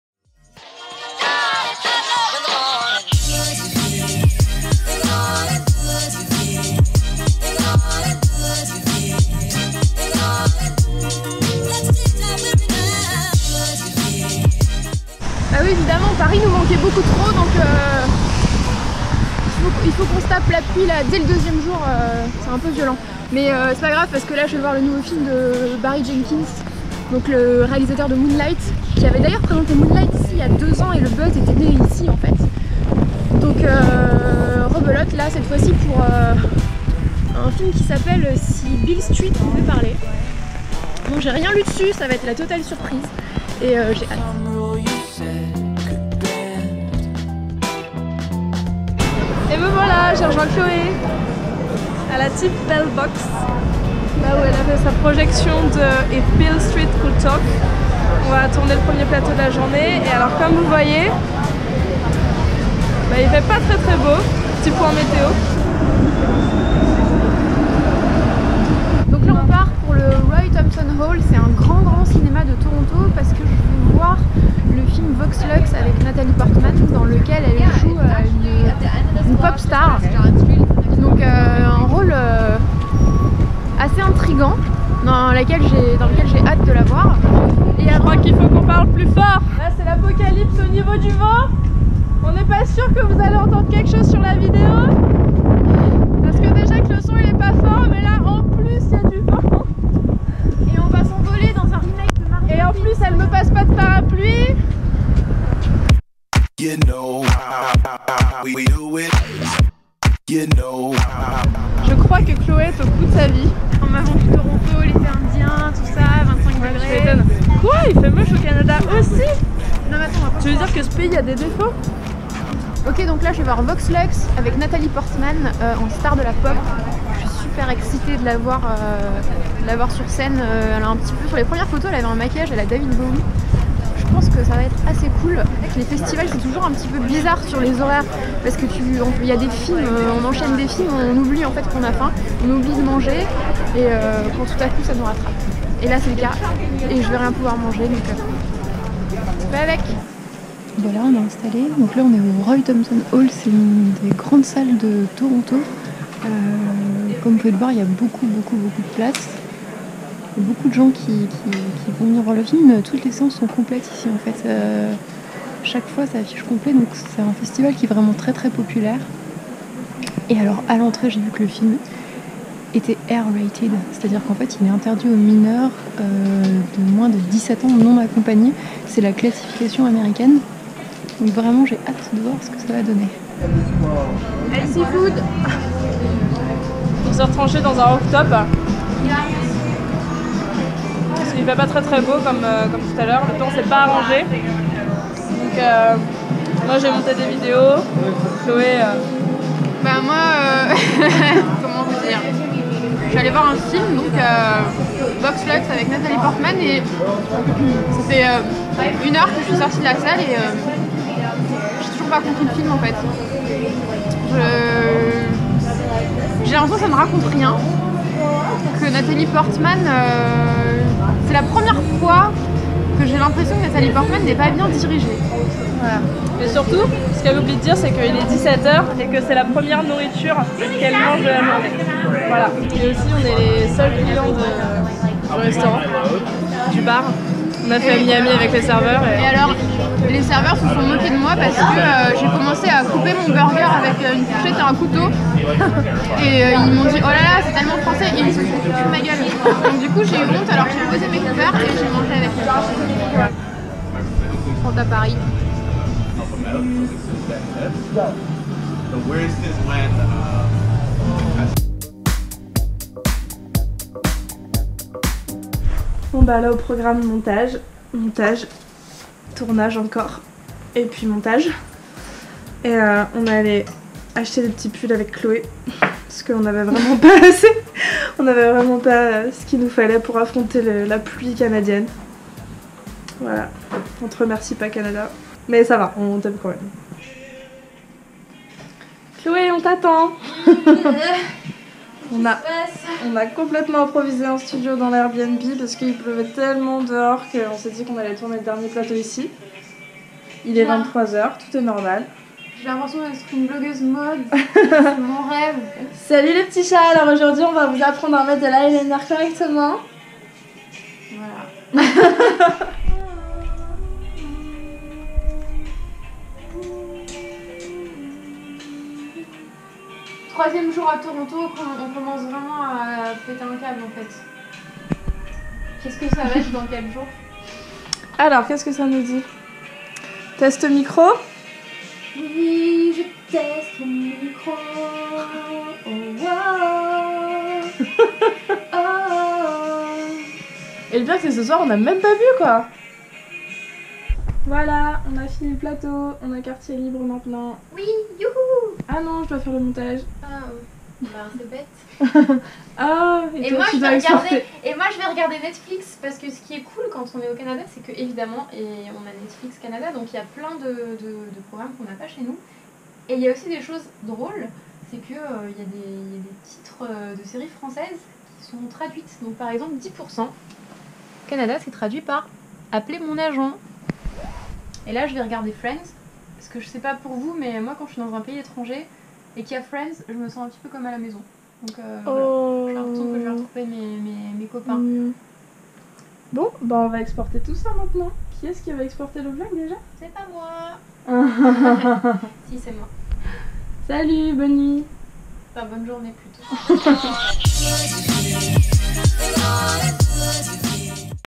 TIFF 2018 : VLOG #2
L'équipe de CinéSérie est à Toronto pour le TIFF (Toronto International Film Festival) !!! Chloé et Morgane vous emmènent dans leurs valises pour découvrir le Festival à travers leurs yeux. Retrouvez toutes nos reviews des films présentés au TIFF 2018